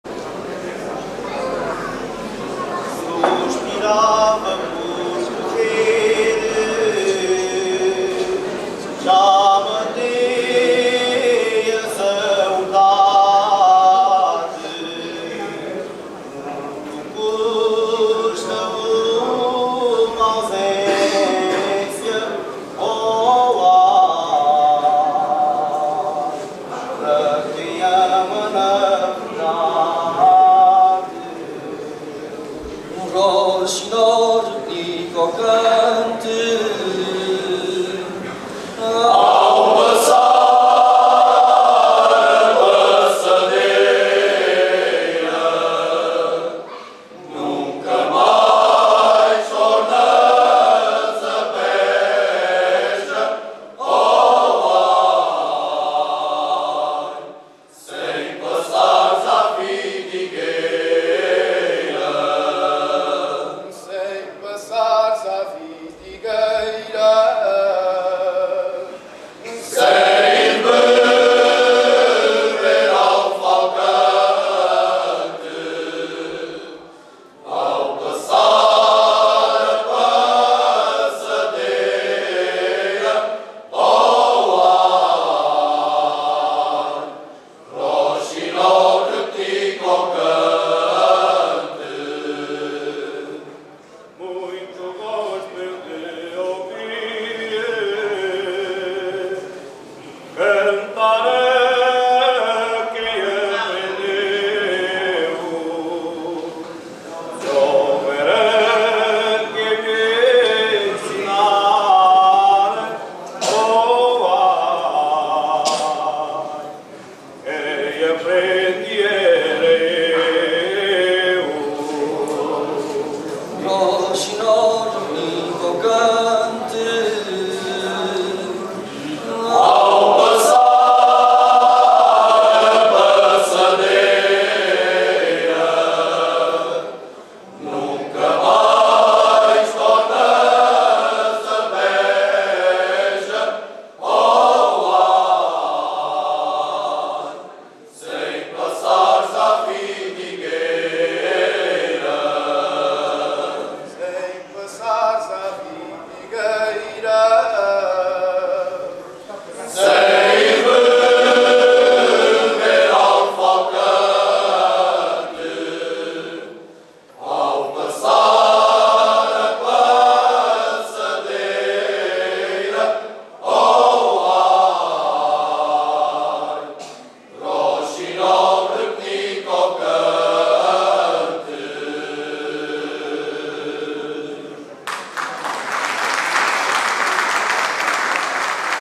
Grupo Coral da Vidigueira
4º Aniversário do Grupo Coral da Vidigueira, Vidigueira - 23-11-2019